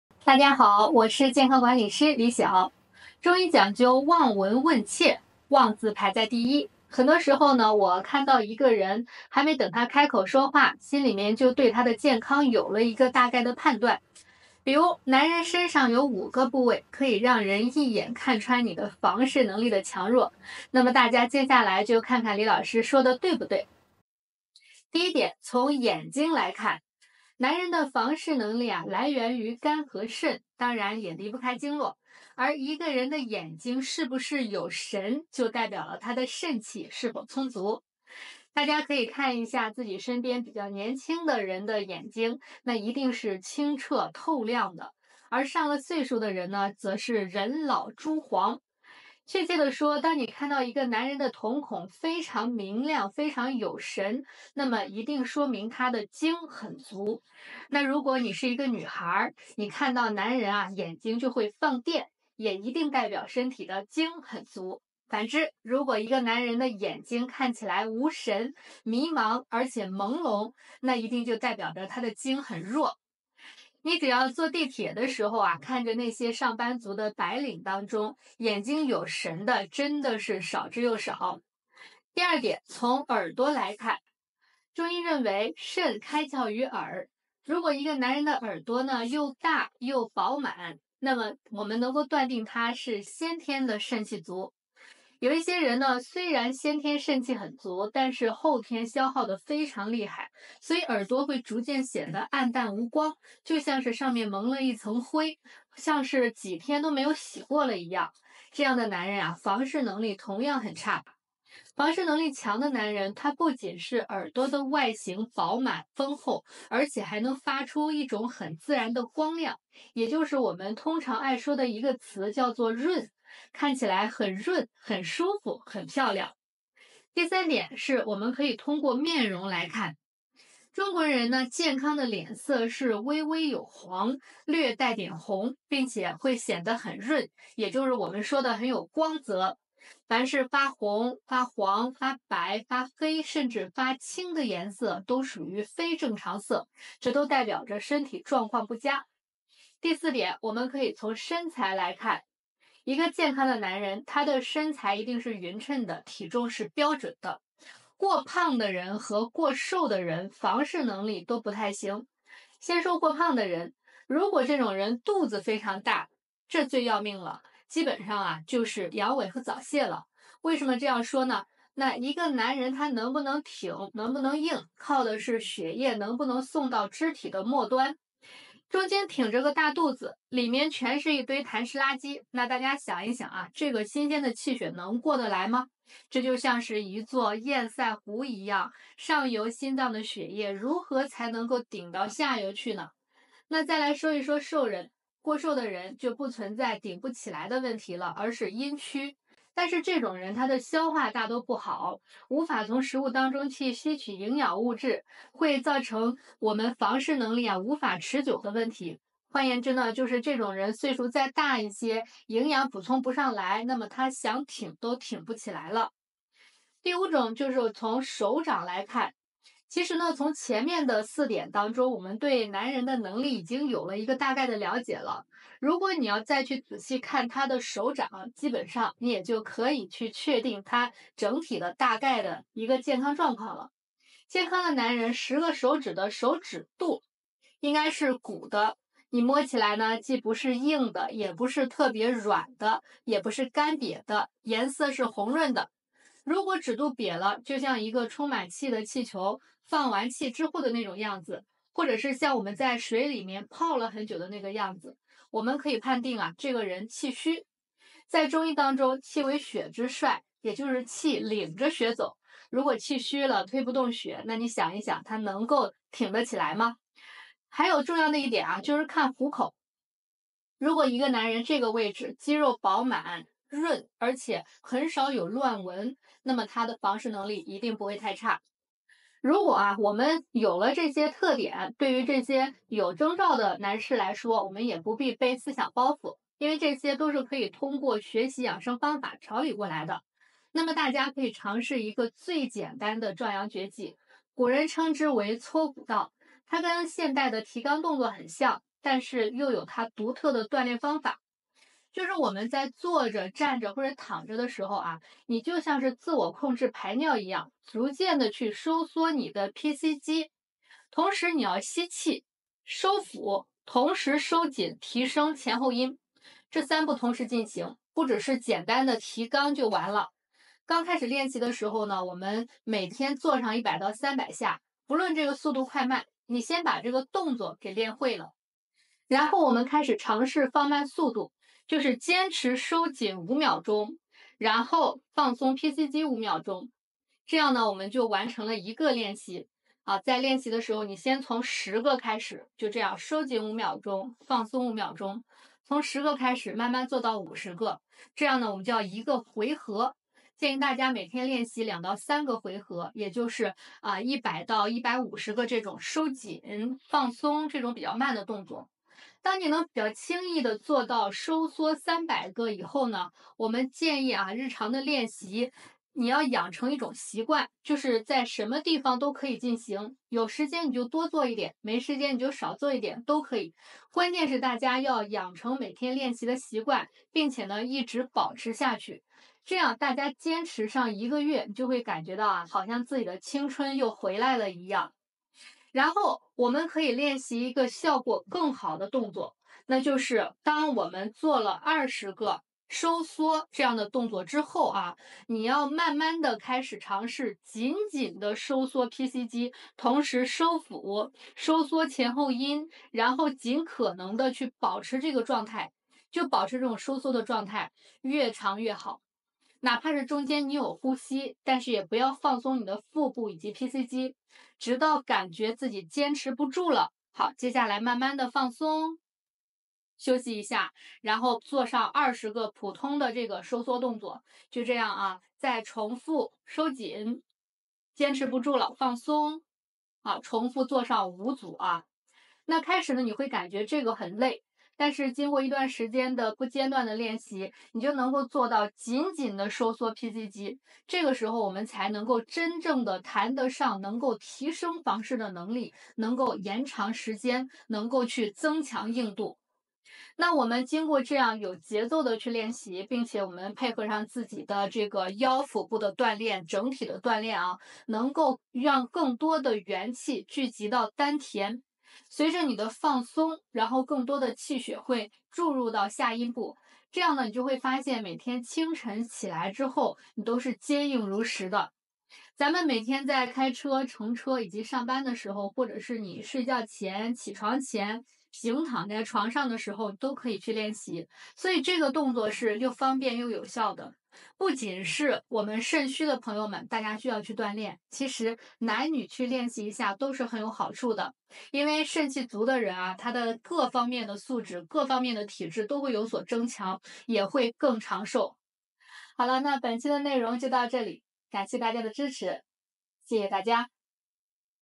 5個部位，一眼看穿男人房事強弱？1招壯陽絕技，30天恢復陽剛！【健康管理師李晓】
一個人的內在健康，會體現在外表上，尤其是男人的腎氣方面，簡直太準了！